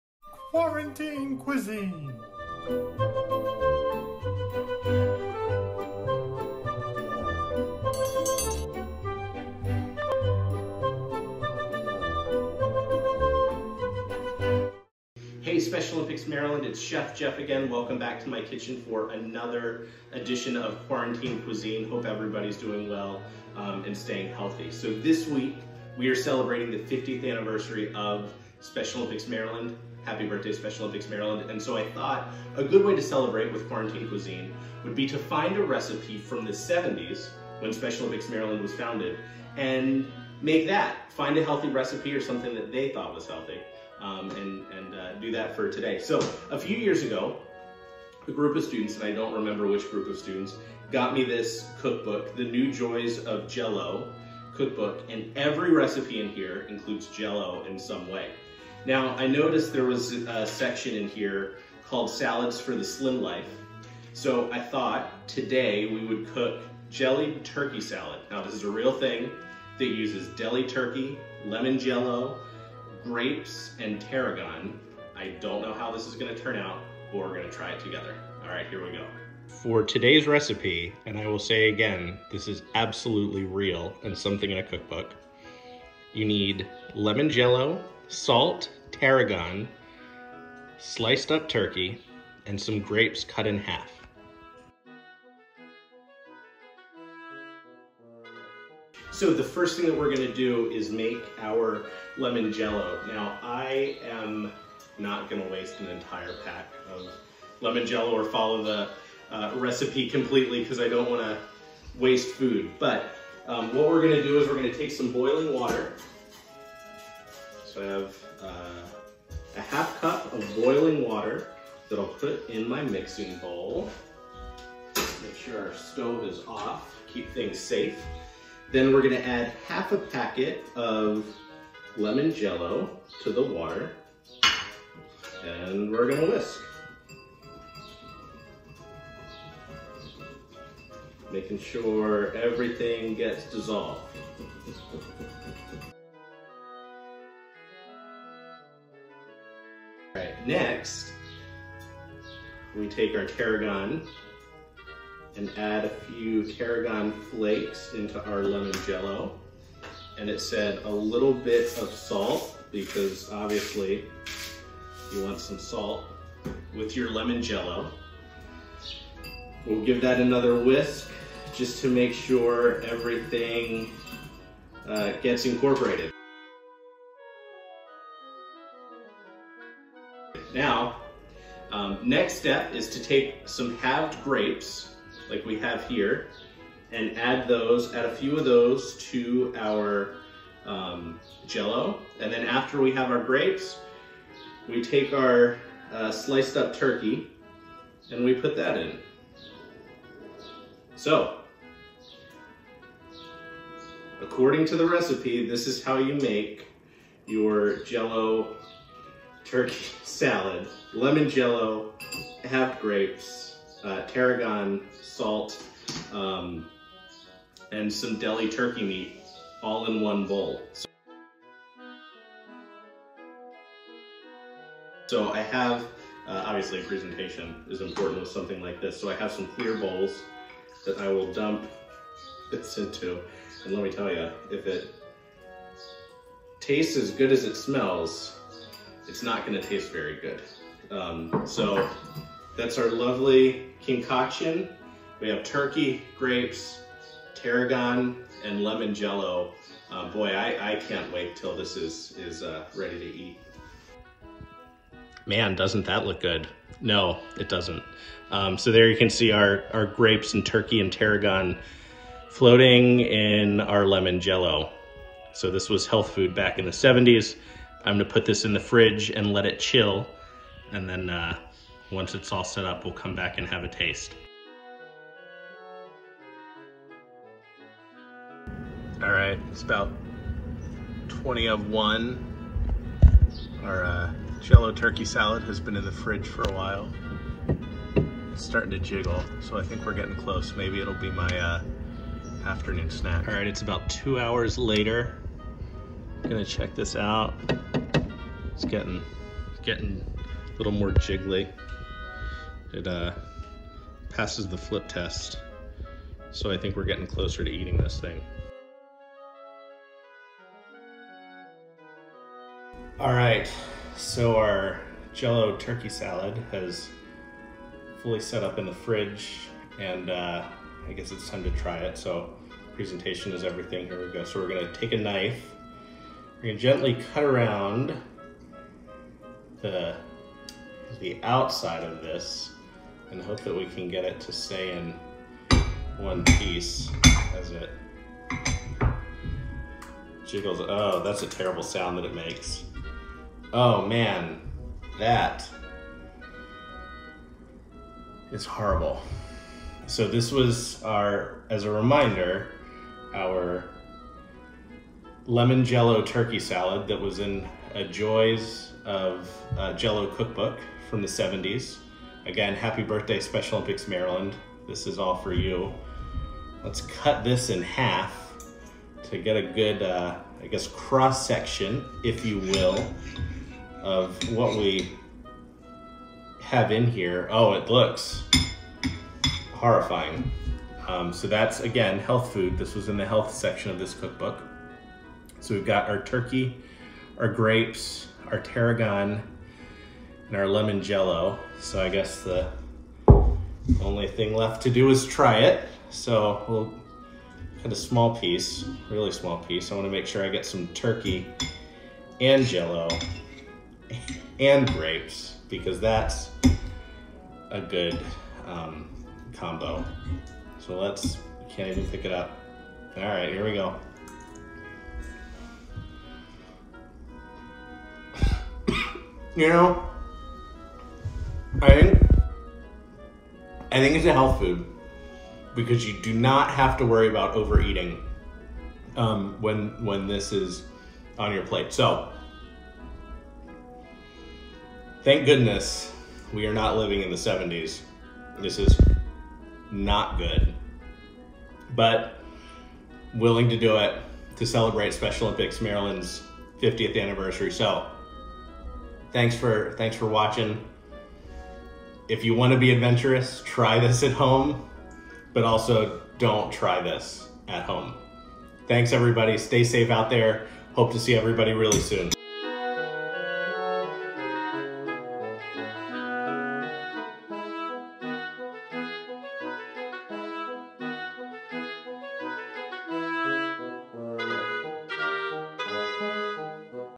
Quarantine Cuisine: 1970's Lemon Turkey Jell-O Salad
Chef Jeff is bringing you a very 70's themed QuarantineCuisine in honor of Special Olympics Maryland's 50th Anniversary. Sweet and Savory never sounded so. well you be the judge. Who's going to try this "far out" meal?